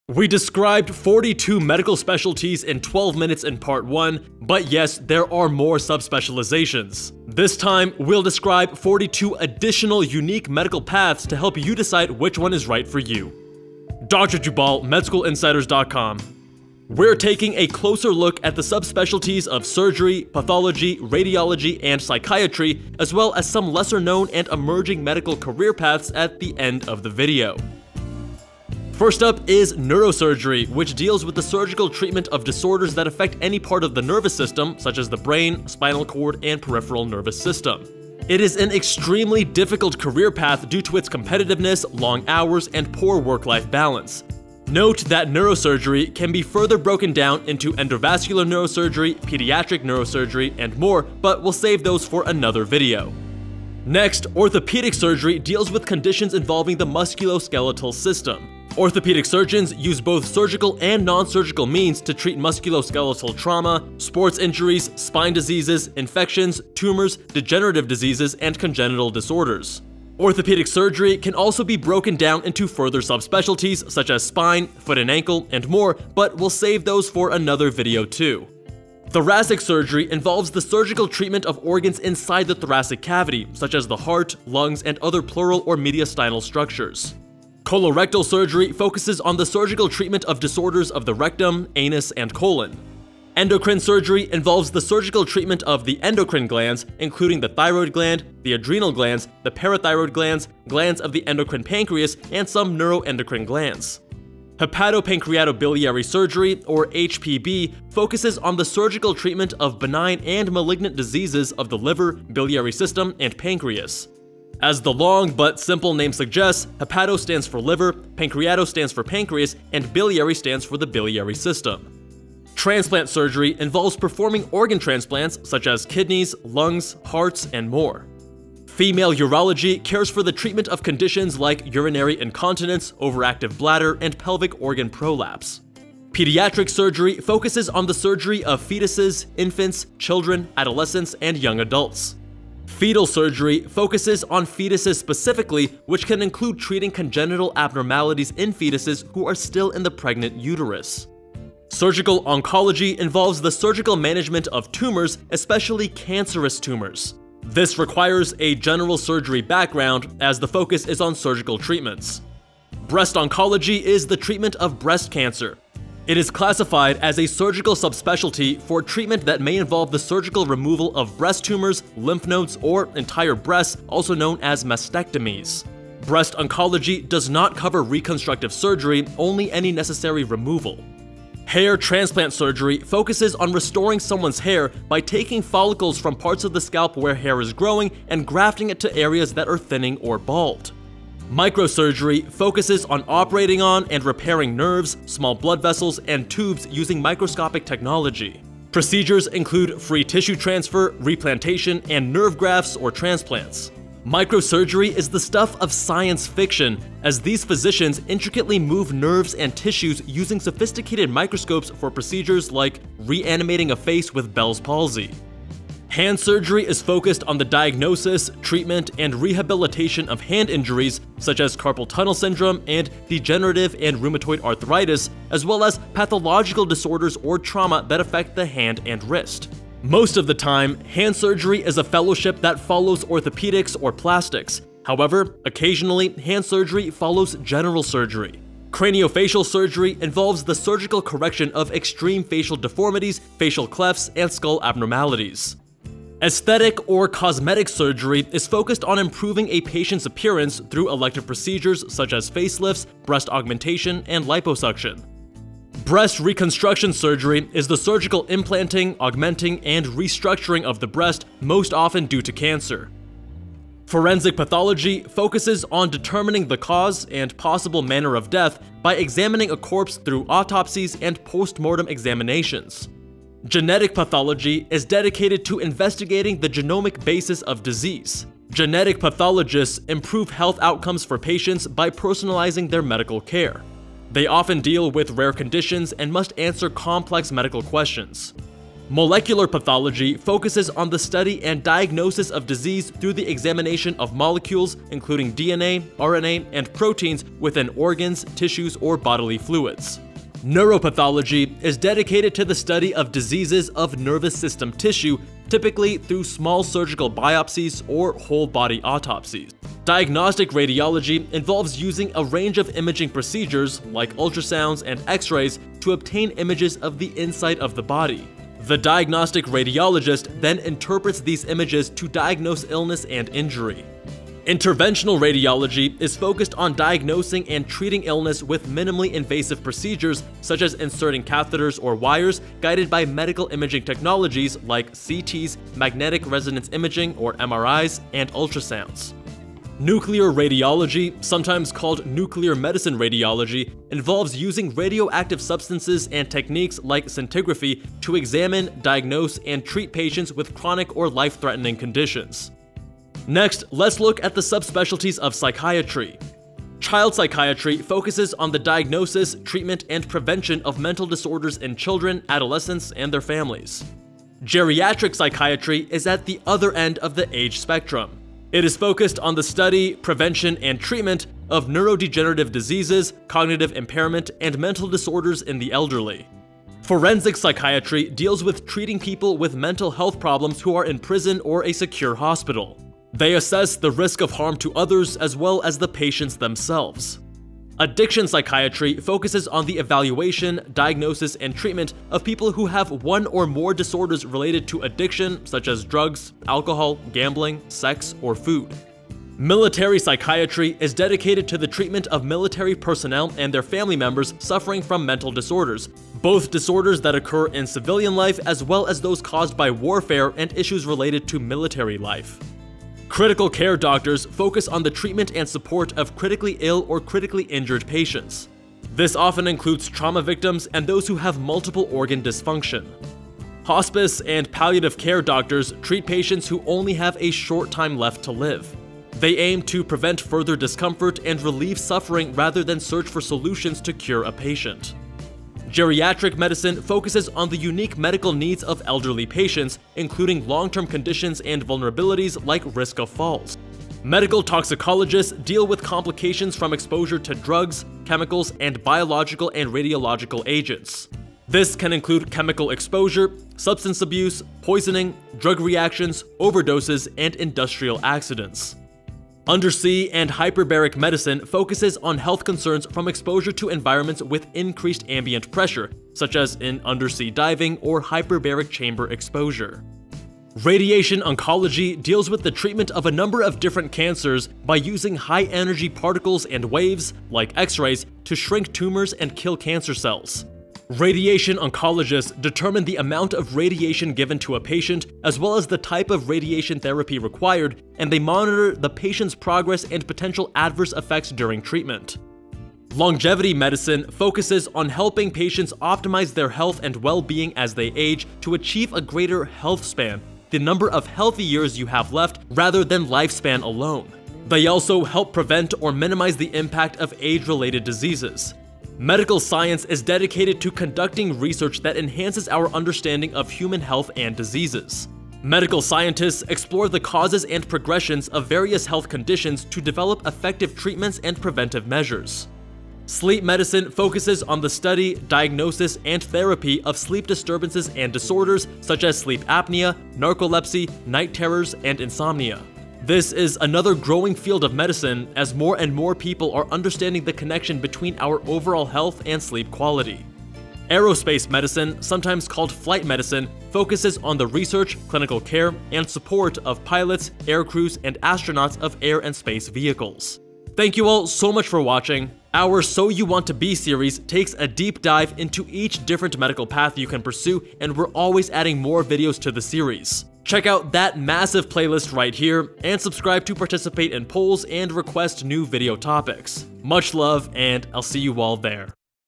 Every Doctor Specialty Explained in 10 Minutes
Welcome to Part 2 of Every Medical Specialty Explained. This time, we’ll describe 42 additional unique medical paths to help you decide which one is right for you.

We’re taking a closer look at the subspecialties of surgery, plastics, pathology, radiology, and psychiatry, as well as some lesser-known and emerging medical career paths at the end of the video.

TIME STAMPS:
00:00 Introduction
00:28 Surgery Subspecialties
04:23 Pathology Subspecialties
05:52 Psychiatry Subspecialties
06:52 Other Medical Careers